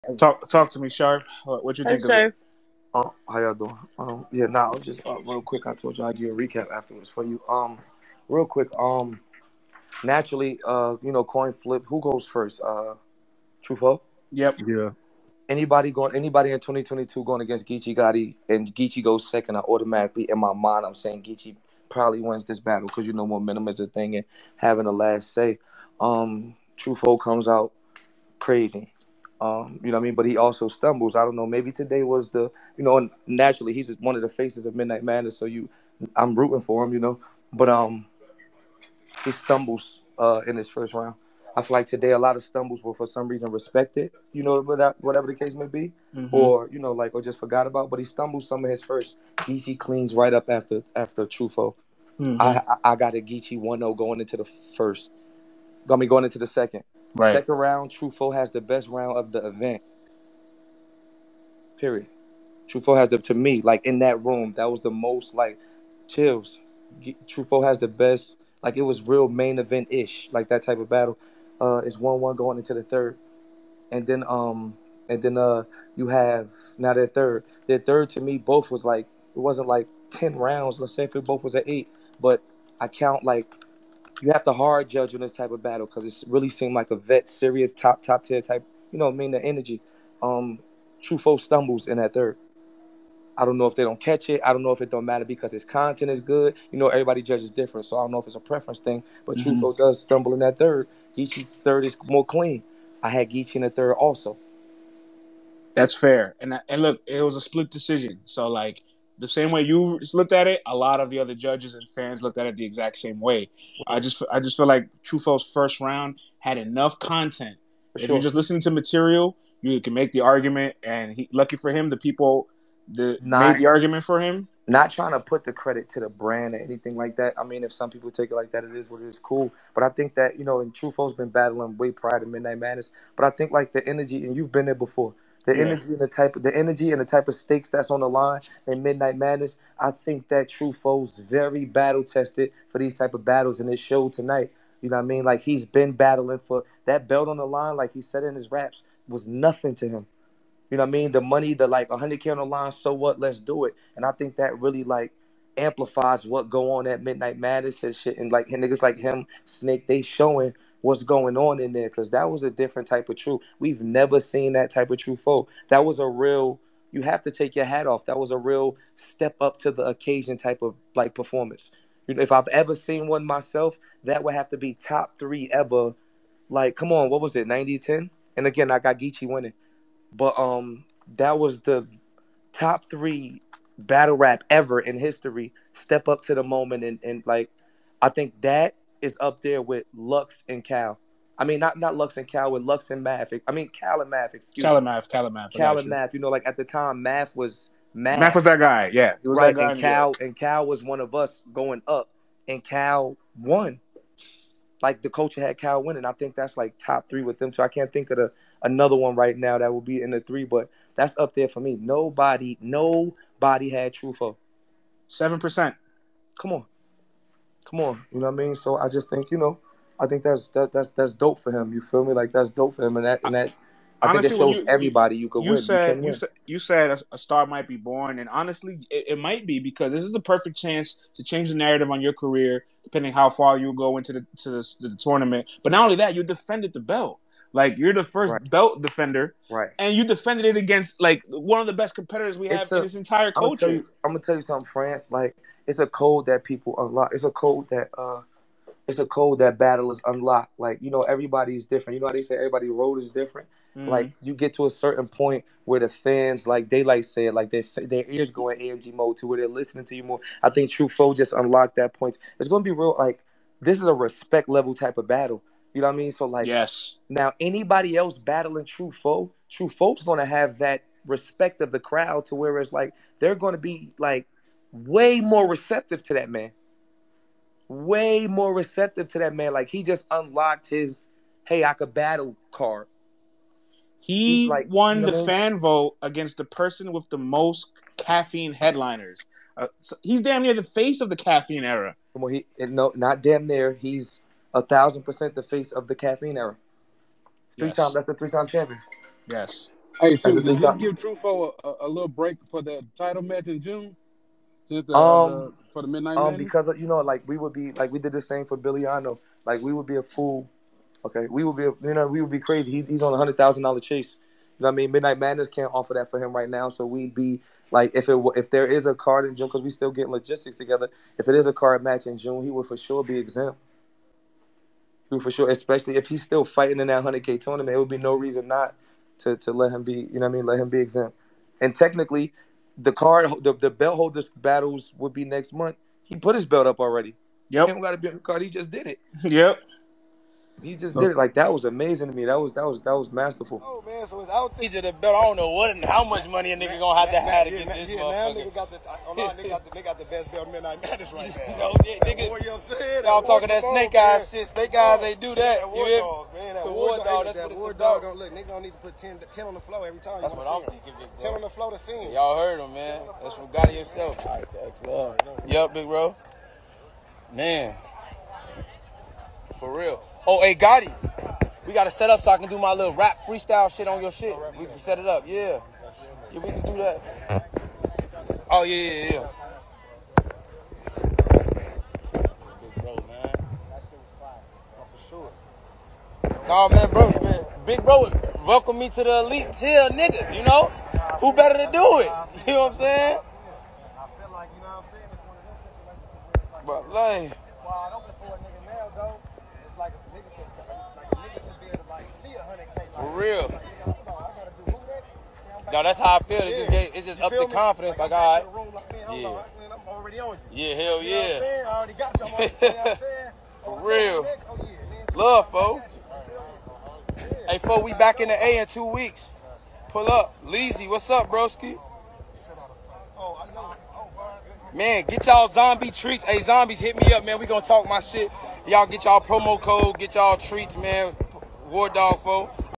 TSU SURF RECAPS TRUFOE VS GEECHI
Surf after ultimate madness recaps Geechi vs trufoe